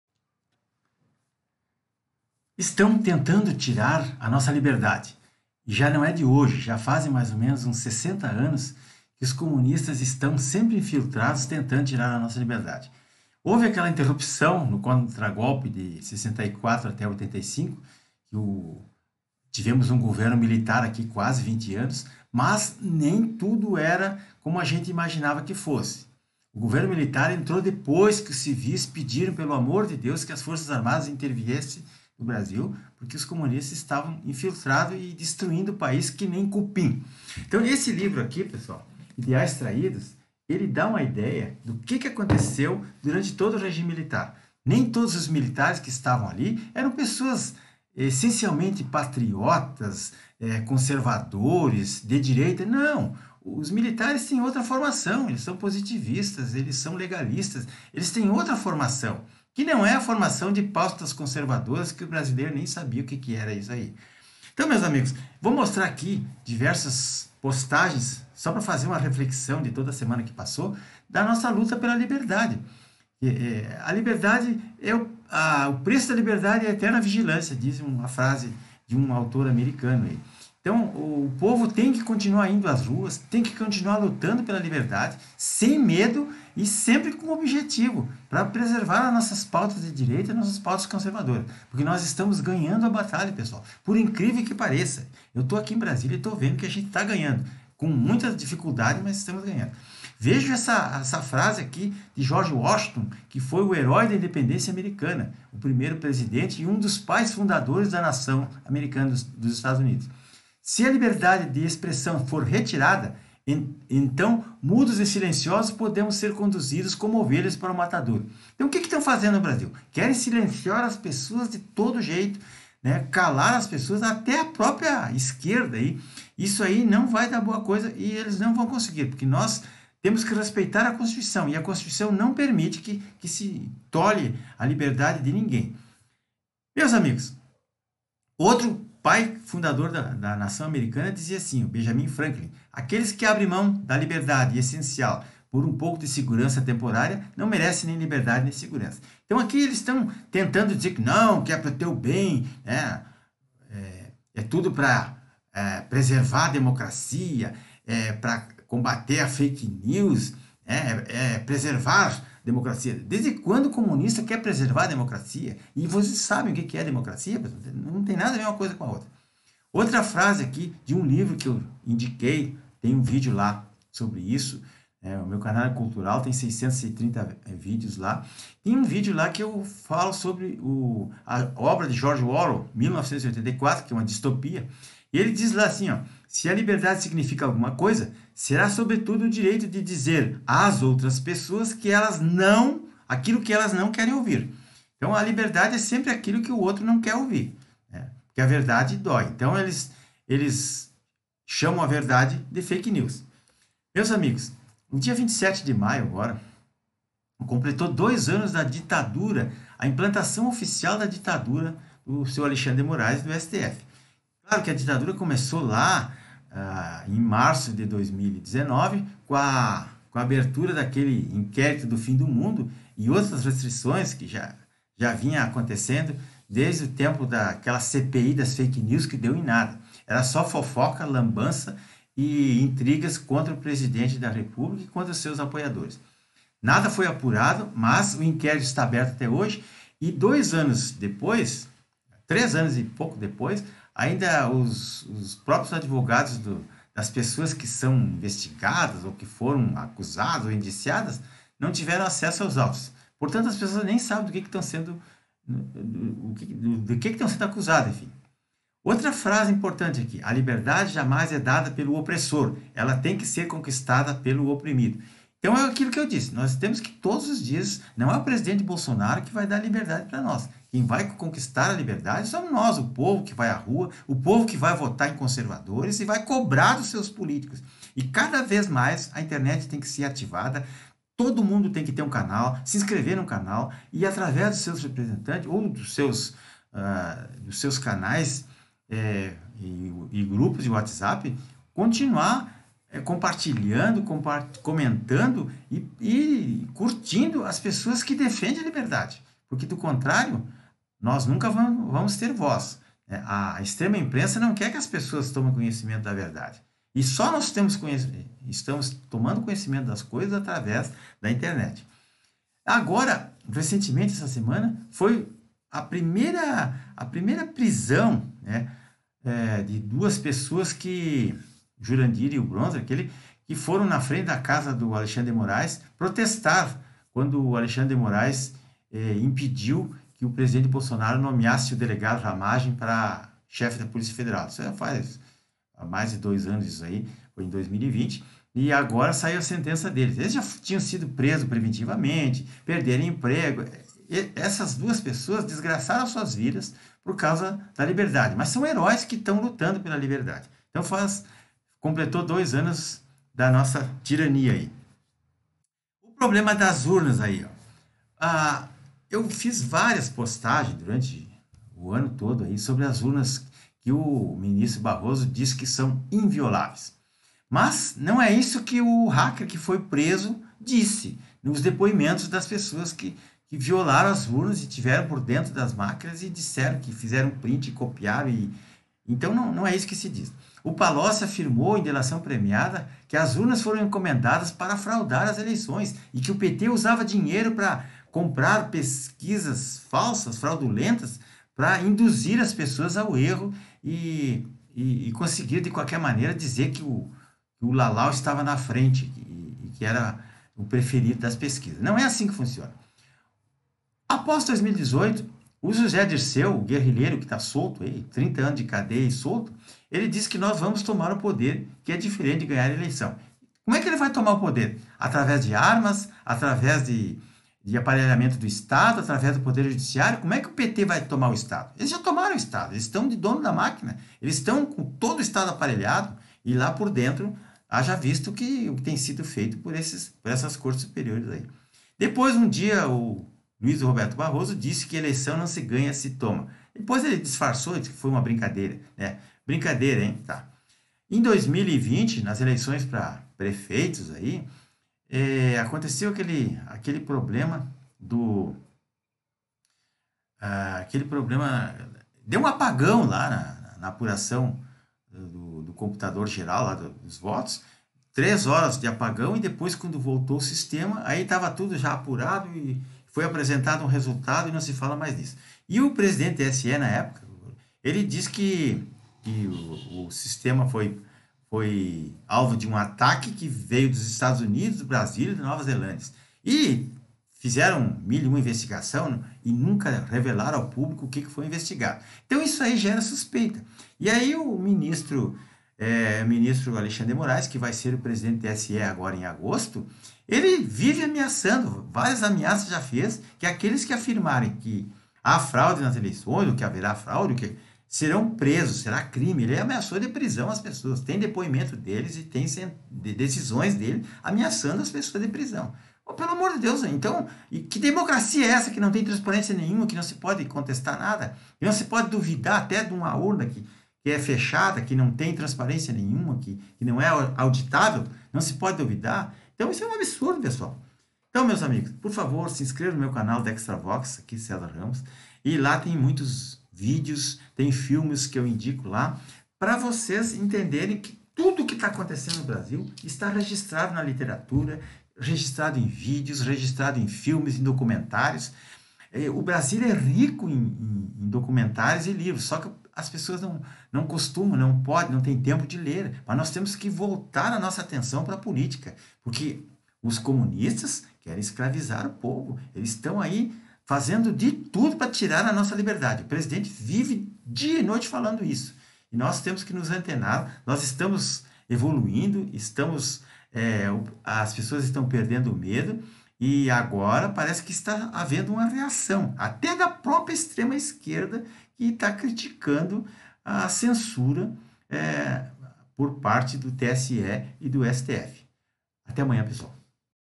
FASCISTAS SÃO ELES
OS VERDADEIROS FASCISTAS ESTÃO TENTANDO TIRAR A NOSSA LIBERDADE ACUSANDO-NOS DAQUILO QUE ELES  SÃO.

LIVROS: 1984, GEORGE ORWELL
IDEAIS TRAÍDOS, GAL. SYLVIO FROTA.